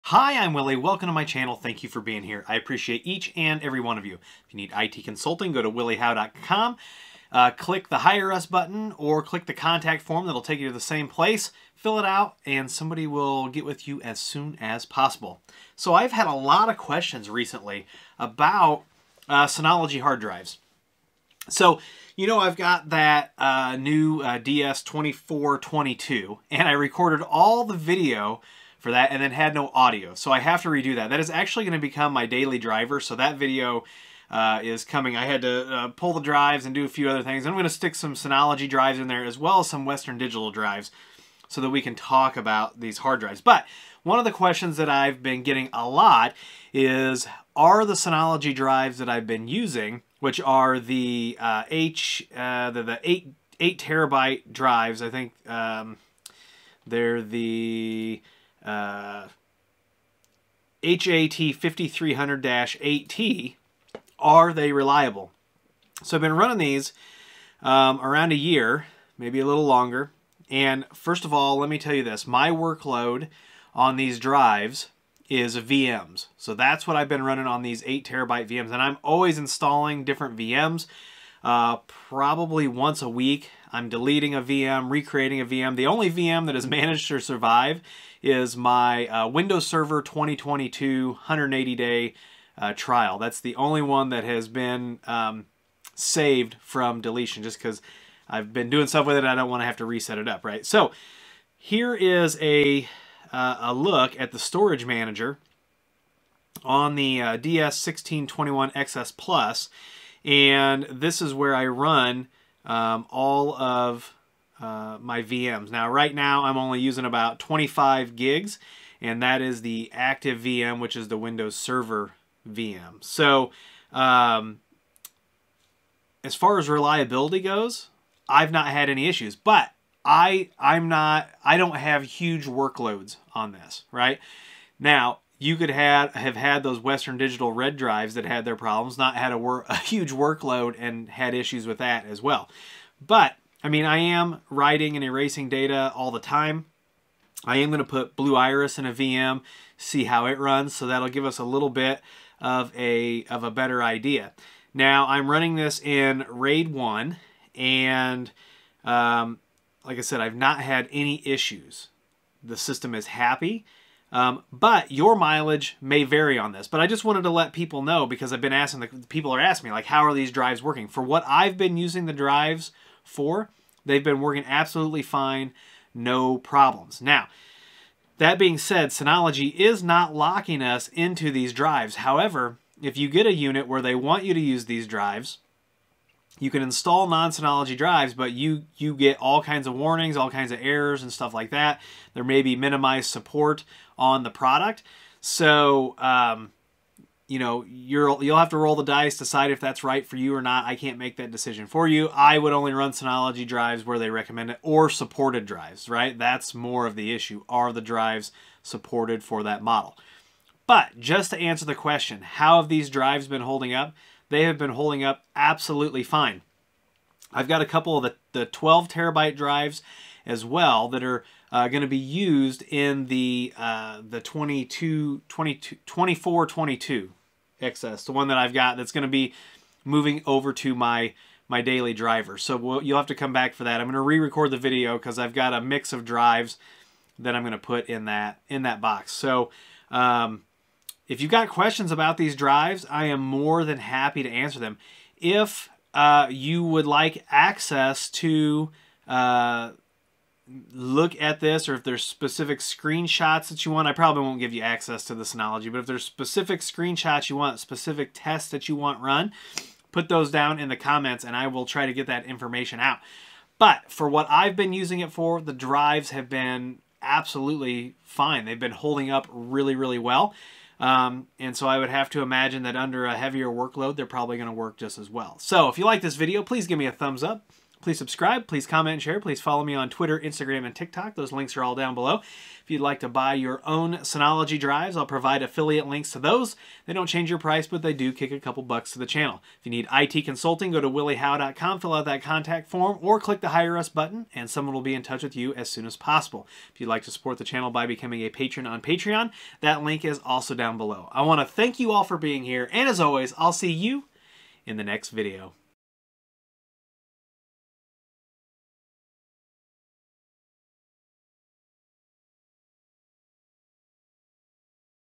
Synology Hard Drive Follow Up - No News Is Good News?
I have had at least a dozen people ask me over the last couple weeks how the Synology Hard Drives that I have are holding up.  Nothing Earth shattering here -- they work well for what I'm doing.  My work load isn't huge -- just some VM creation/deletion.  Your mileage may vary!

Time Stamps:
00:00 - Intro
00:20 - Where's the DS2422+ Video at??
01:00 - 8TB Model
01:15 - My workload on the drives -- likely light
01:50 - Storage Manager with an active VM
02:00 - Are they reliable?
02:35 - RAID Level
02:50 - How are they working?
03:00 - Non Synology Drives on certain models or non supported drives
03:40 - They are holding up fine
04:10 - What else do you want to know about the drives?
04:40 - Wrap Up